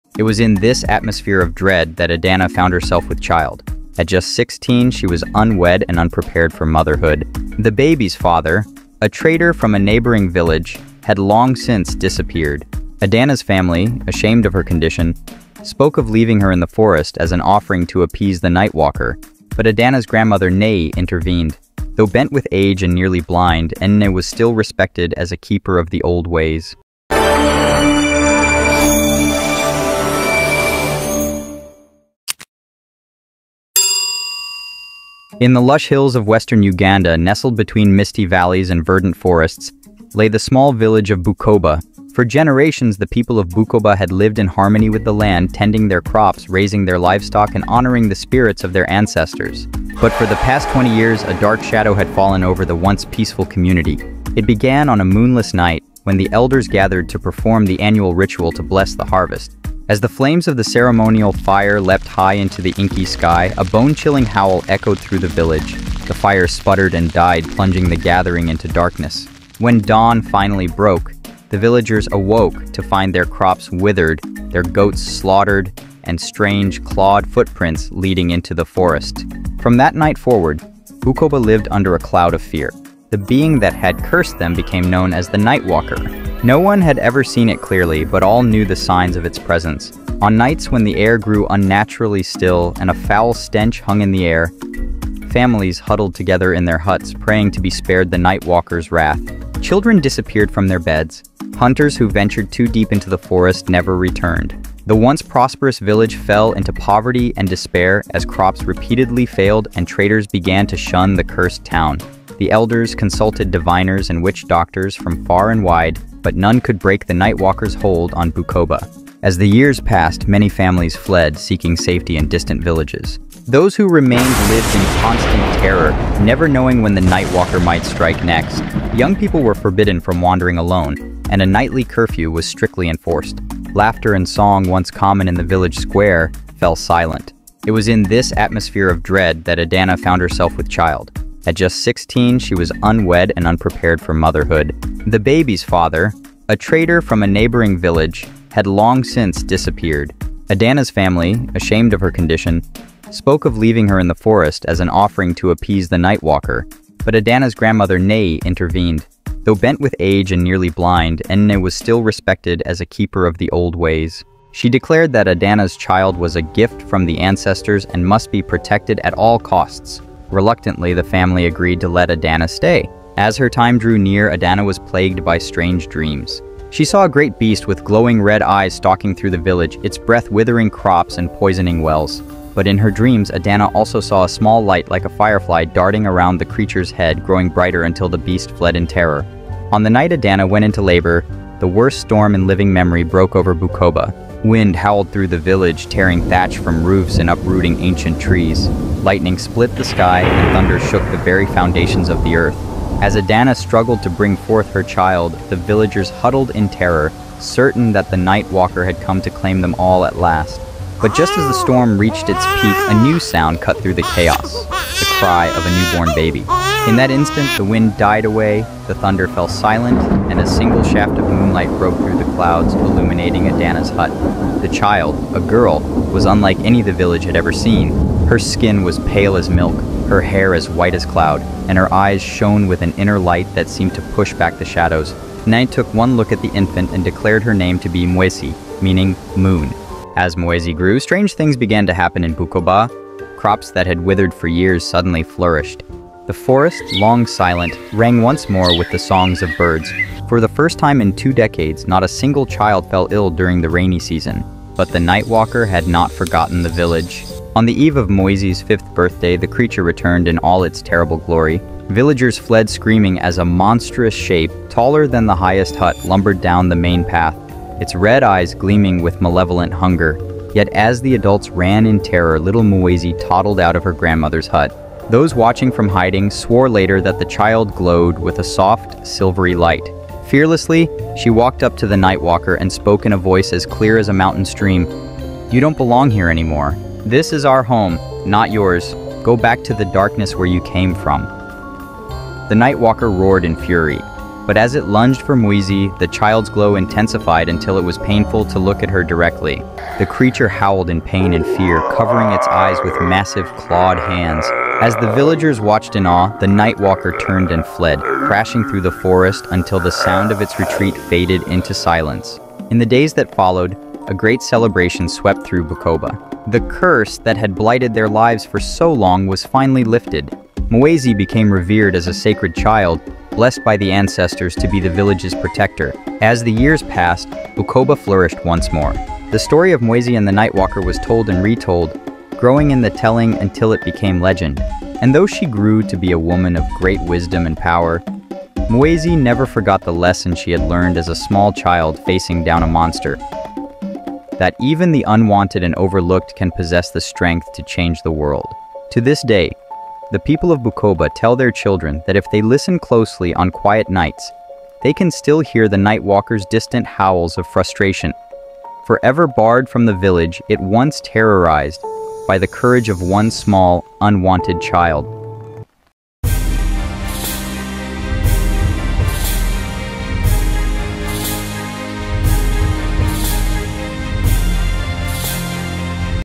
The Unwanted Baby | African Folktale
Discover the enchanting tale of an unwanted child who becomes the savior of her village. This African story weaves together themes of fear, hope, and the power of the human spirit. Watch as we bring to life the legend of Mwezi, the albino girl born with the ability to banish an ancient evil plaguing her community.

In this video, we explore:
• The curse of the Night Walker and its impact on the village of Bukoba
• The birth of Mwezi, an unexpected hero
• How an outcast child becomes the village's protector
• The triumph of light over darkness

Perfect for lovers of African folklore, magical realism, and inspiring stories of overcoming adversity.